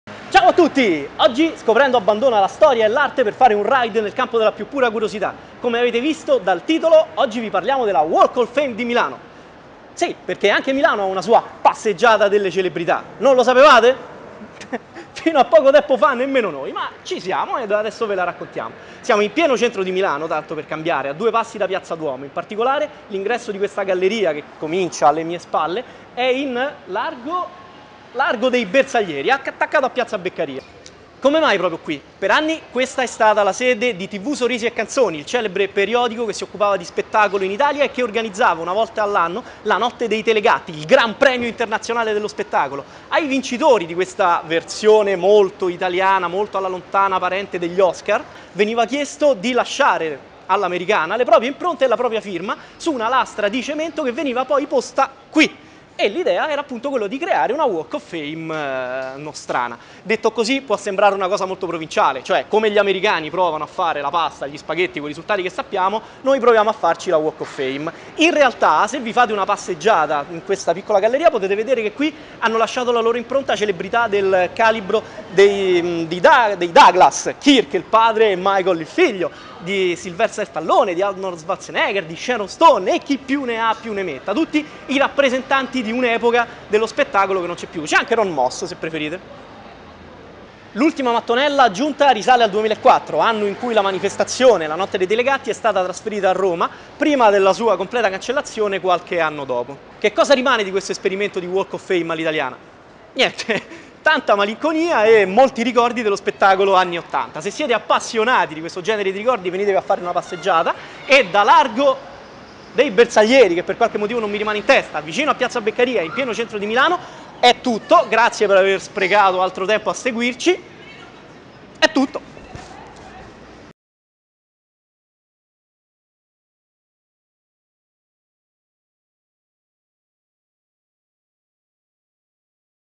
[10] La "Walk of fame" di Milano
Molti divi di fama mondiale hanno lasciato la propria impronta in questo luogo, a disposizione dei curiosi. Le circostanze lo hanno fatto dimenticare, ma la posizione (a due passi dal Duomo,in pieno centro di Milano) rende agevole fargli una visita, non solo per gli appassionati del mondo dello spettacolo...